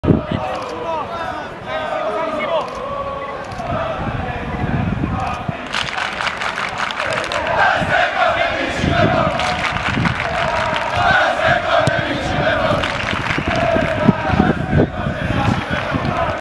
L'Aquila-Benevento 2-2, Lega Pro 2013-2014 (5)
Allo Stadio "Fattori" di L'Aquila i padroni di casa subiscono una brusca frenata nella corsa alla Serie B venendo raggiunti nei minuti finale dal Benevento. Sugli spalti discreta presenza di pubblico con gli ultras di casa che sostengono con buona costanza i propri giocatori ed il centinaio di tifosi ospiti che alterna buoni momenti di tifo a cori di contestazione.